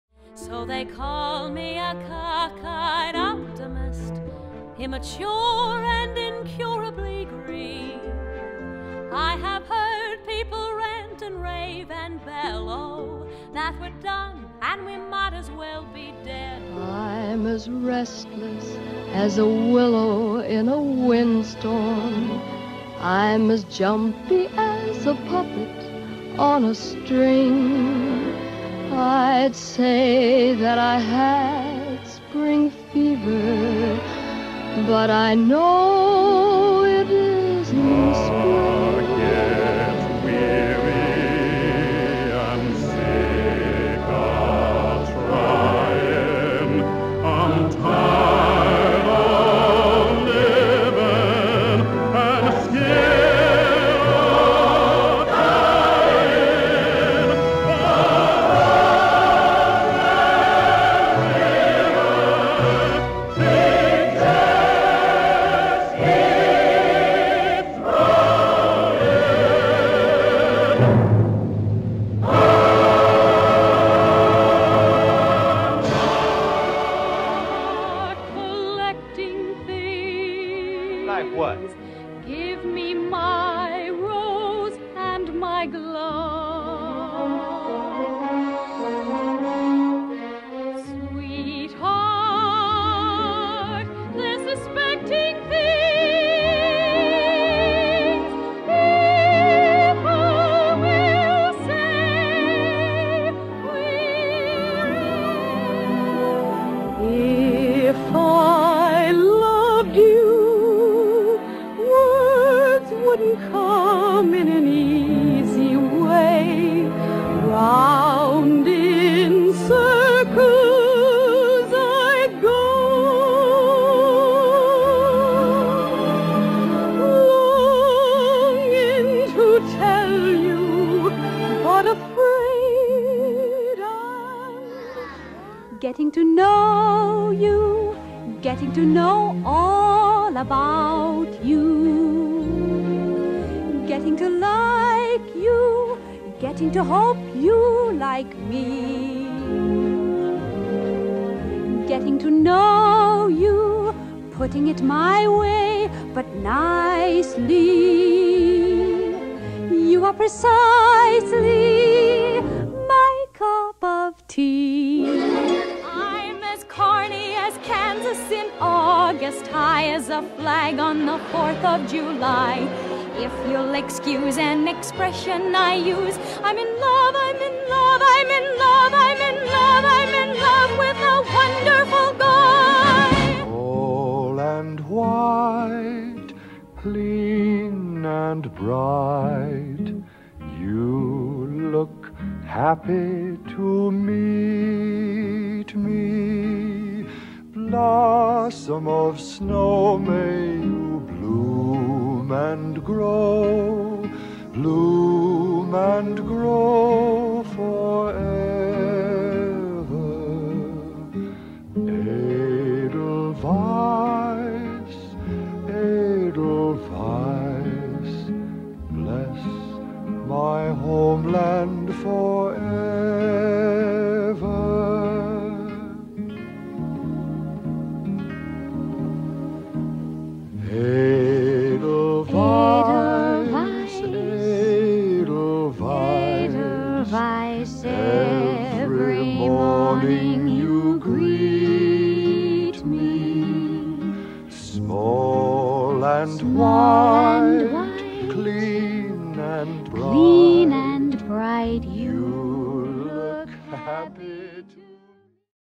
A Tribute to Oscar Hammerstein II
Today, we celebrate Oscar Hammerstein II with this special tribute video!